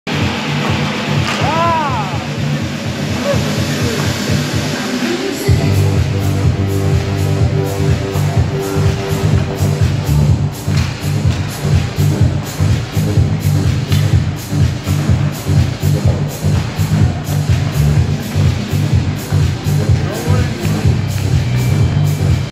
Rock paper scissors
Austin taking on petr stepanek from Peterborough phantoms ice hockey at rock paper scissors.